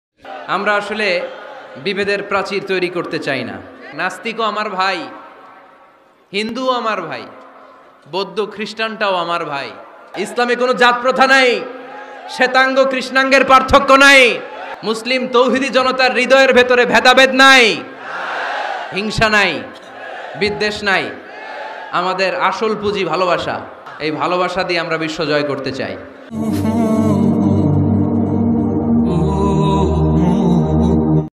হিন্দু মুসলিমদের নিয়ে একি বললেন মিজানুর রহমান আজহারী
Islamic waz, mizanur rahman azhary waz,short waz, all bangla waz, all bangla tieps, Mehedi Hasan